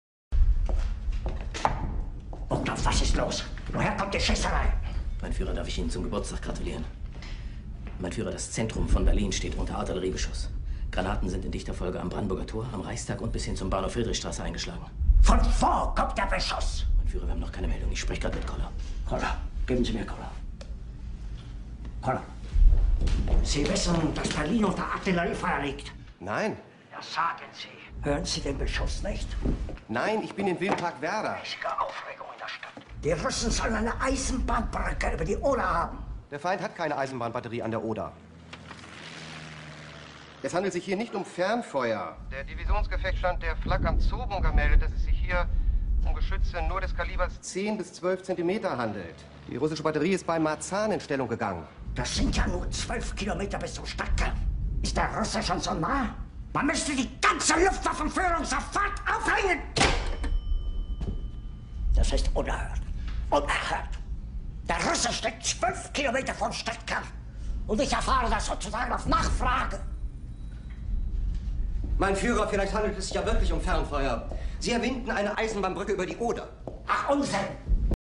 Hitler phones about his breakfast ( Read Description )
Just to show you guys I still have the moves.

* School just started for me, so I might make videos when dismissal and weekends.